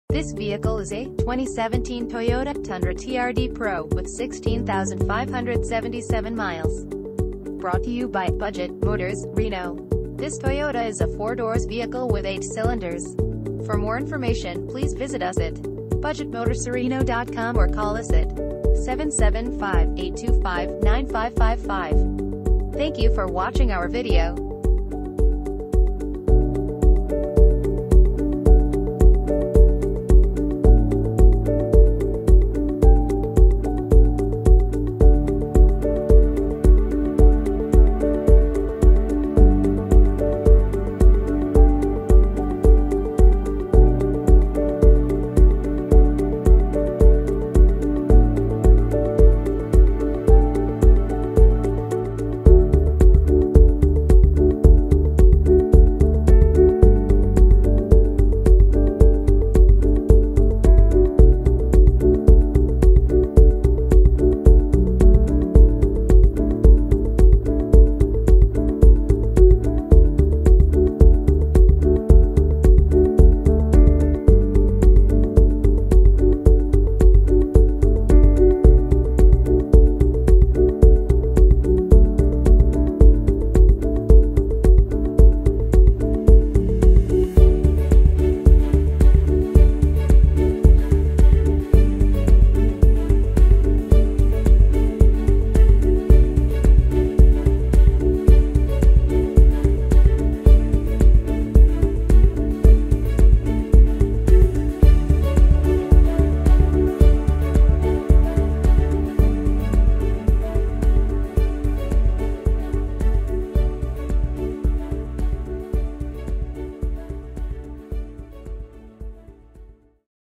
2017 Toyota Tundra - Budget Motors Reno
Budget Motors Reno
3155 Kietzke Lane

2017 Toyota  Tundra

Clean title, 16,577 miles, Automatic, Pickup, 4 Wheel Drive, 4 doors, 8 cylinders, MPG City 13 and 17 in Hwy.